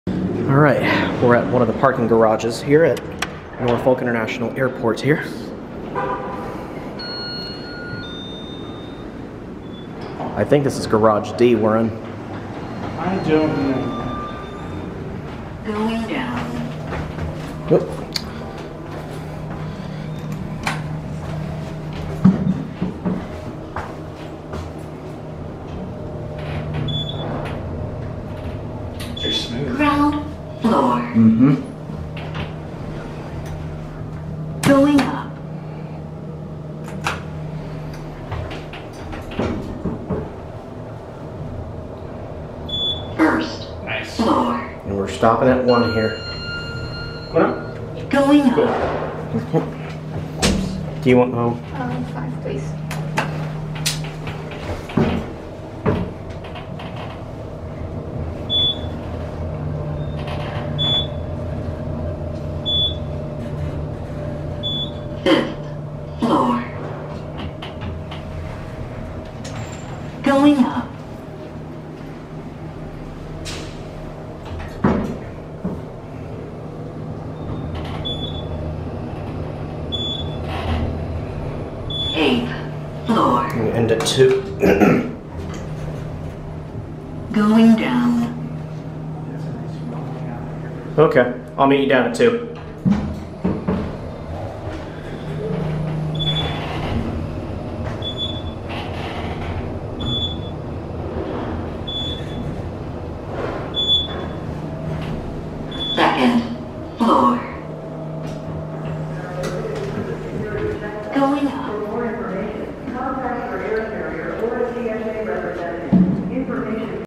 Otis Traction Garage D Elevators At Norfolk International Airport (With Evan Stone Productions)
Riding the Parking Garage D elevators at Norfolk International Airport in Norfolk VA I was riding and filming with Evan Stone Productions